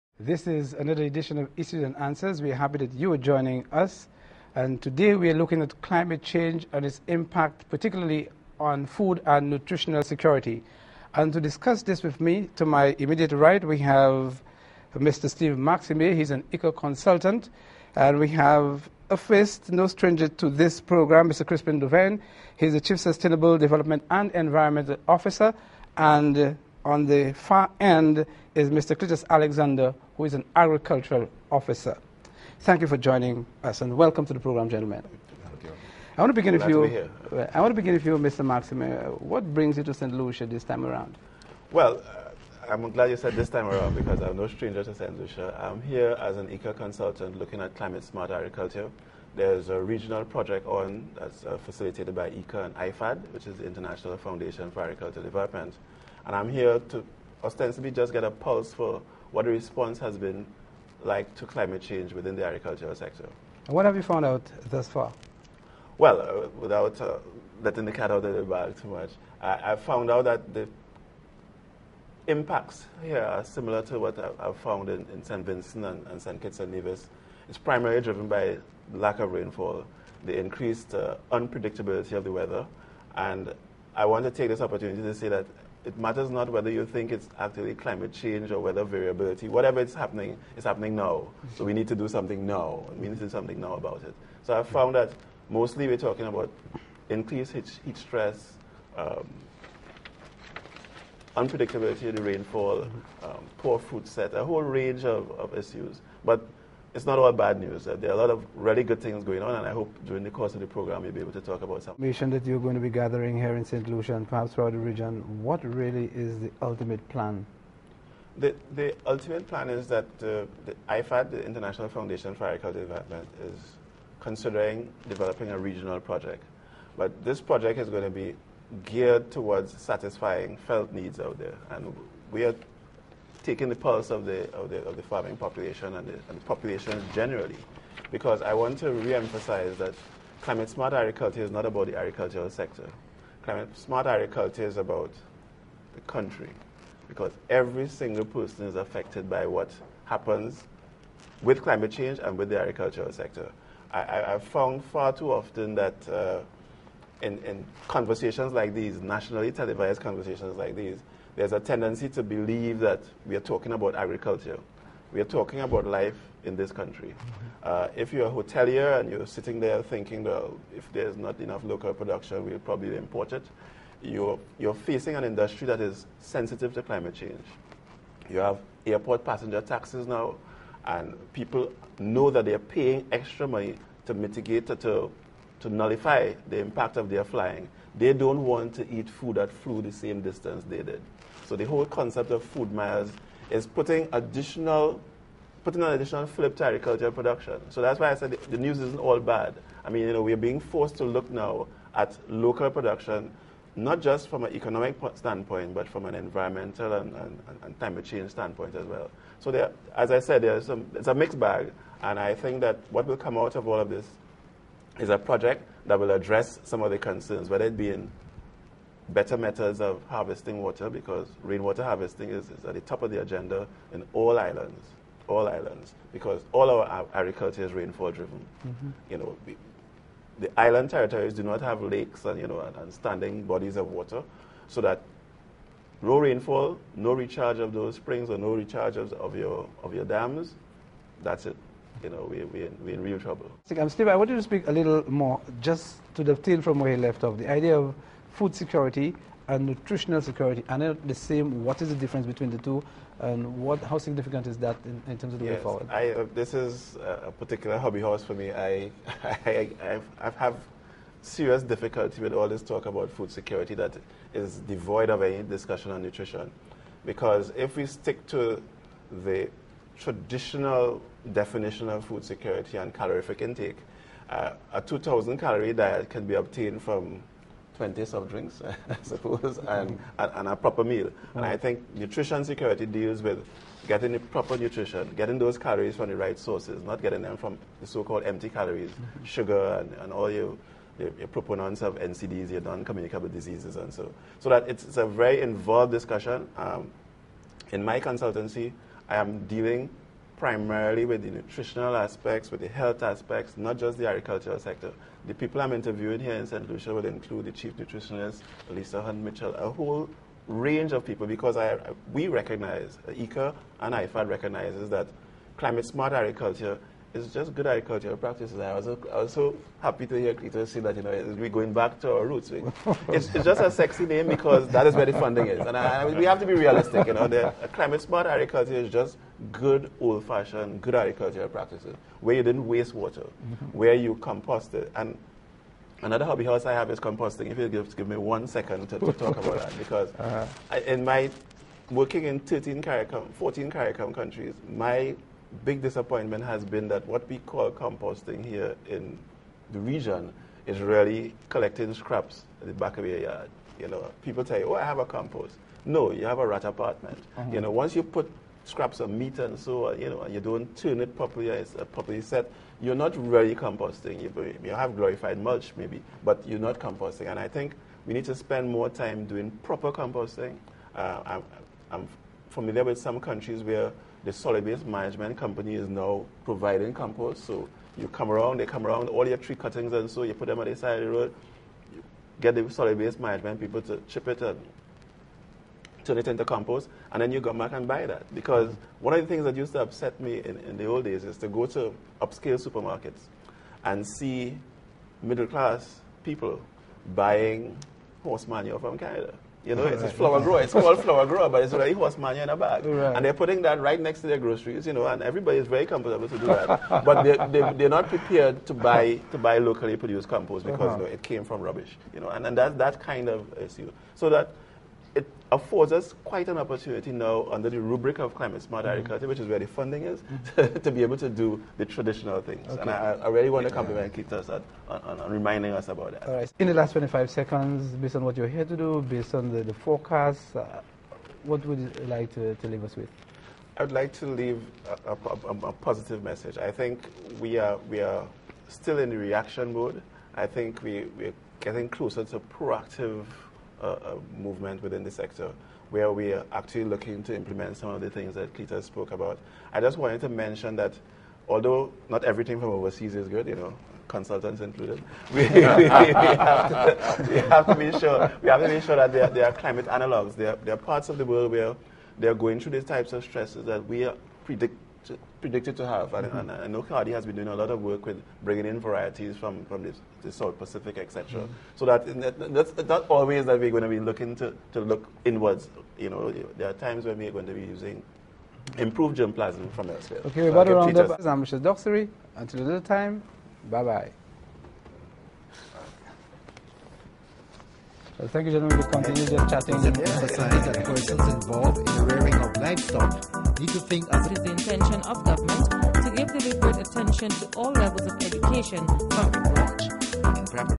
St Lucia Climate-smart agriculture
Televised interview featuring the contribution of IICA Consultant Steve Maximay